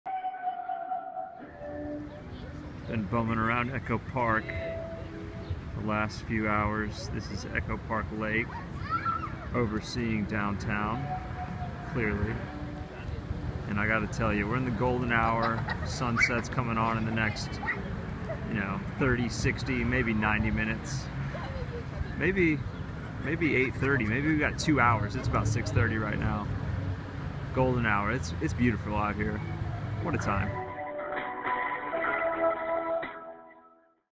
Golden Hour Echo Park
Lil quickie of a beautiful evening in Los Angeles